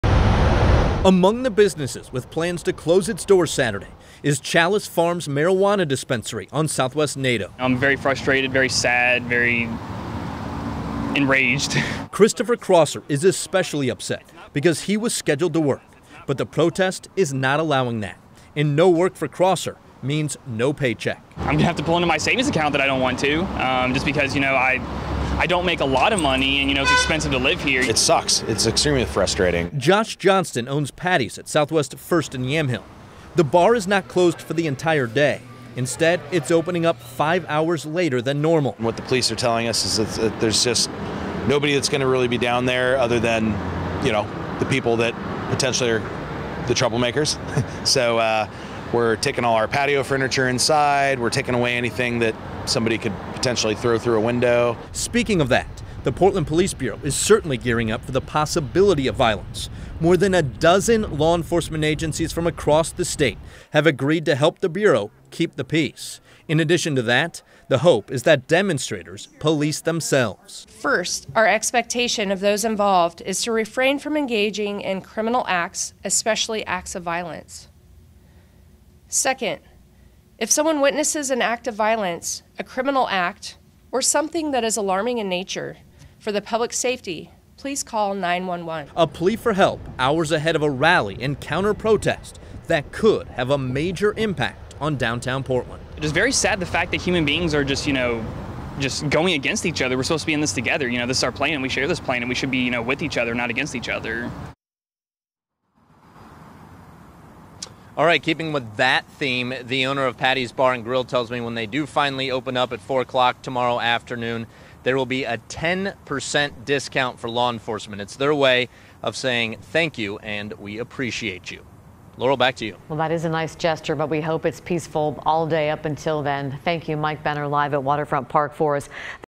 Businesses closing due to planned protests in Portland
Numerous businesses have announced that they will be closed Saturday due to dueling protests between right-wing and left-wing groups.